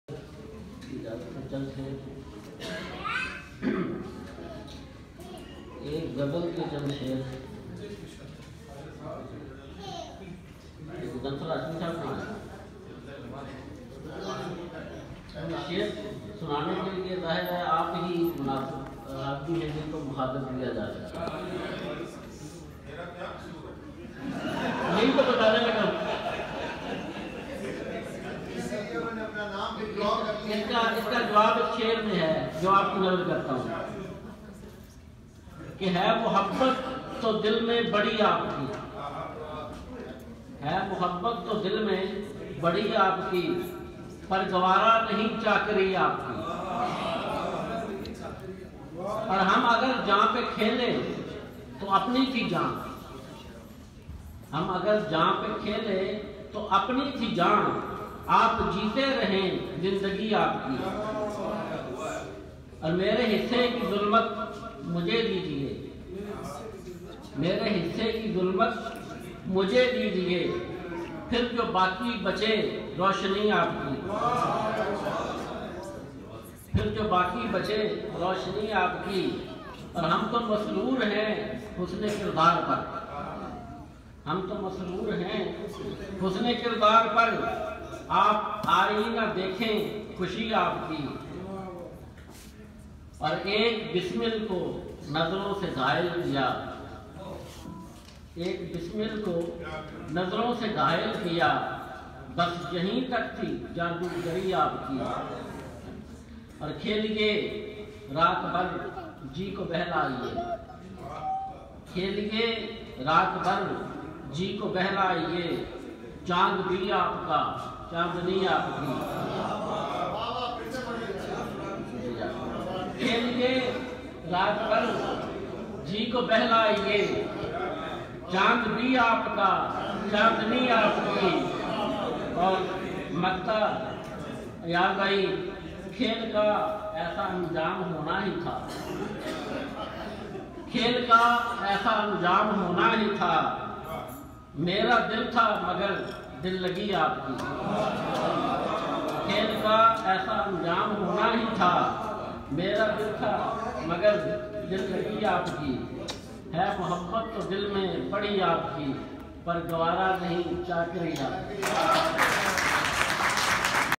Altaf Bukhari reciting his poetry
Chai shai restaurant
Syed Ayaz Mufti kee kitab kee runmai kee taqreeb
Houston USA 01132018